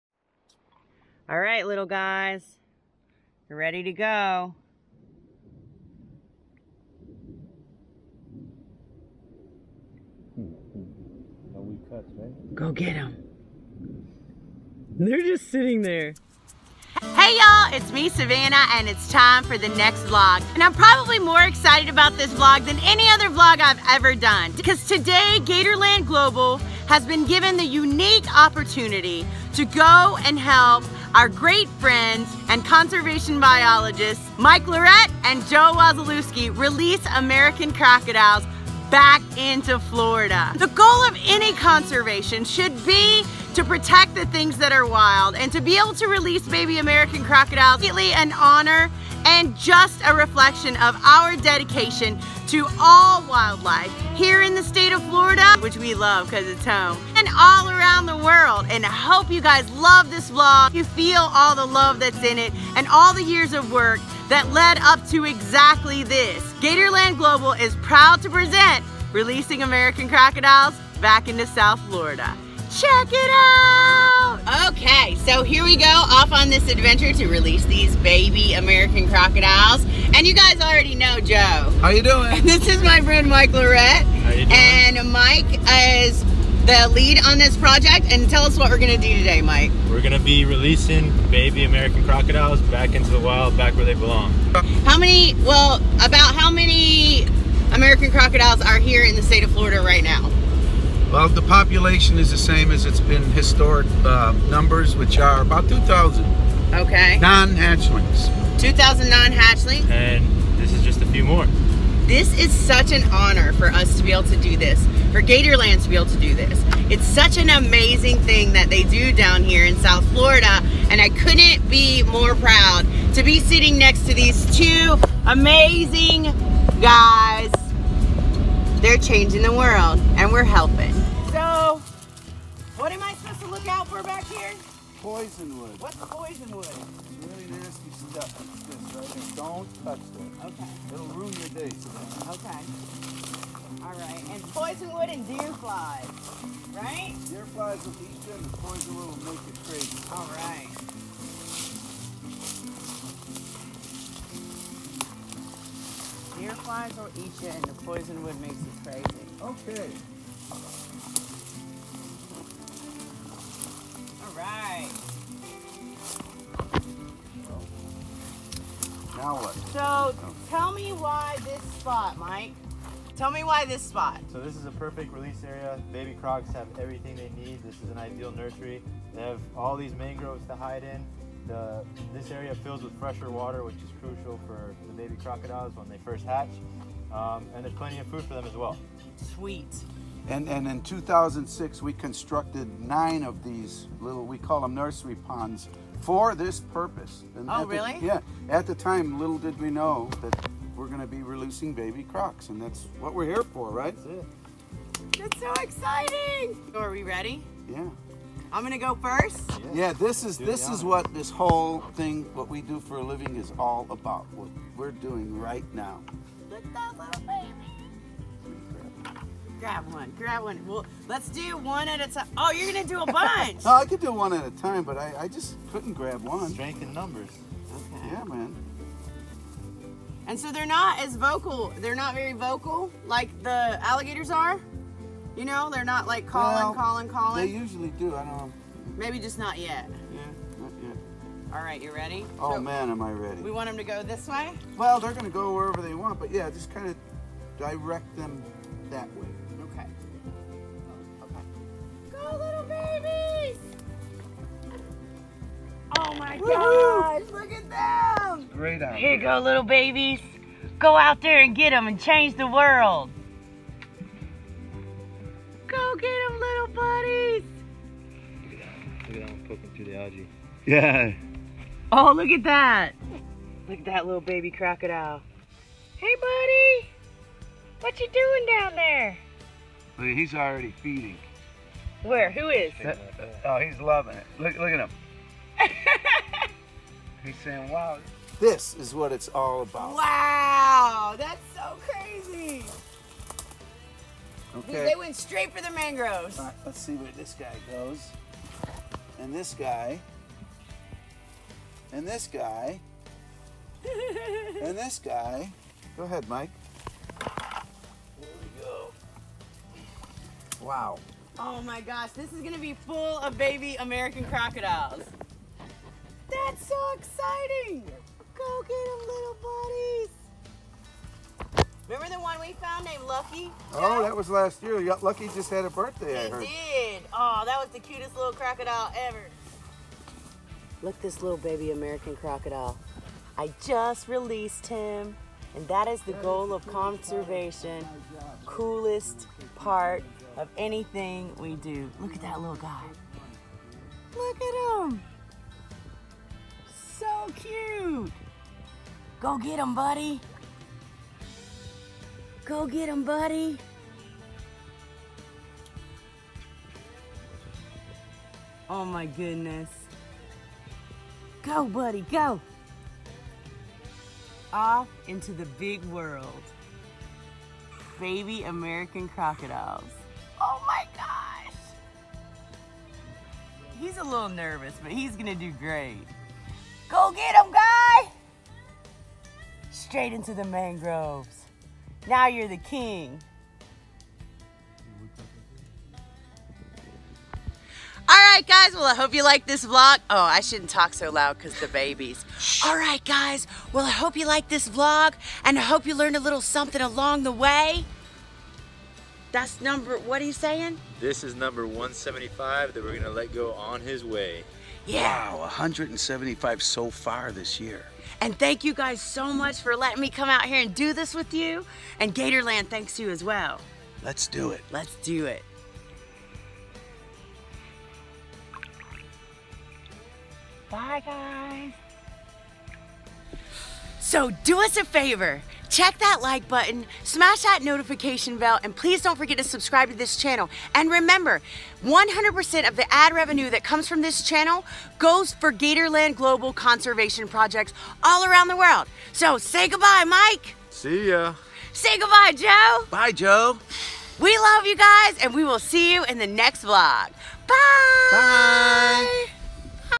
Releasing Baby American Crocodiles!
American Crocodiles live naturally in South Florida but they are in the the threatened species list.  Go with Savannah & Joe Wasilewski & Mike Lloret deep into beautiful South Florida to help release some beautiful baby American Crocodiles!